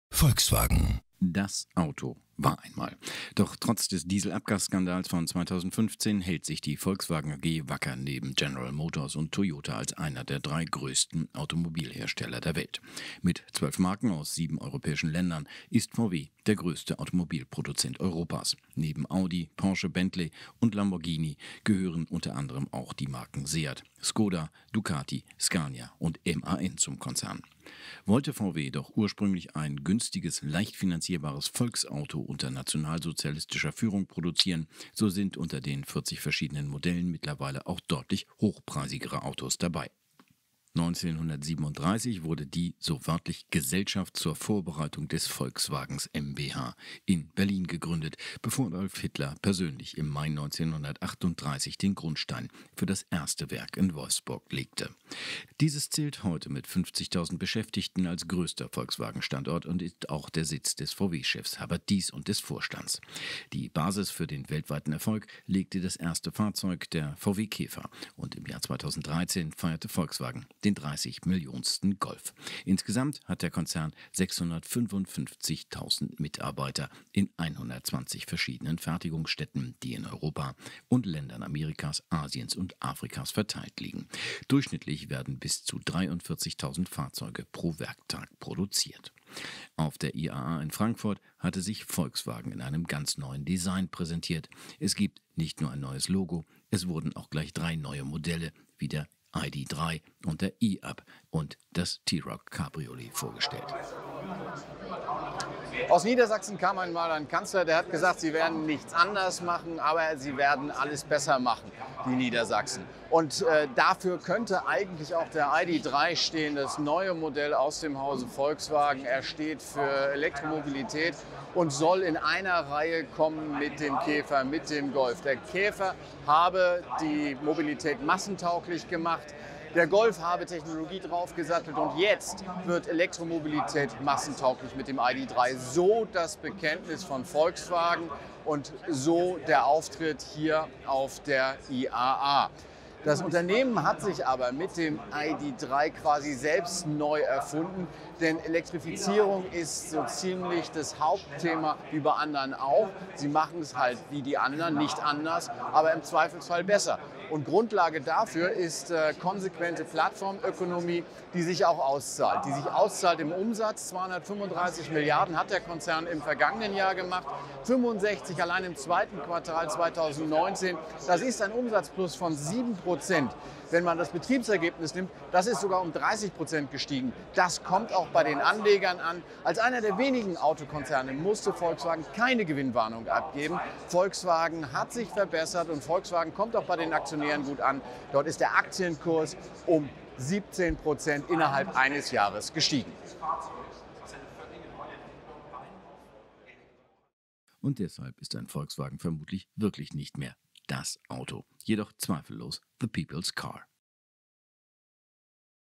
Volkswagen Aktie I Mit Elektro-Motor die Krise besiegen?
„Das Auto“ war einmal. Doch trotz des Dieselabgasskandals von 2015 hält sich die Volkswagen AG wacker neben General Motors und Toyota als einer der 3 größten Automobil-Hersteller der Welt.

Mit 12 Marken aus 7 europäischen Ländern ist VW der größte Automobilproduzent Europas! Neben Audi, Porsche, Bentley und Lamborghini gehören unteranderem auch die Marken Seat, Skoda, Ducati, Scania und MAN zum Konzern. Gevestor stellt ihn vor.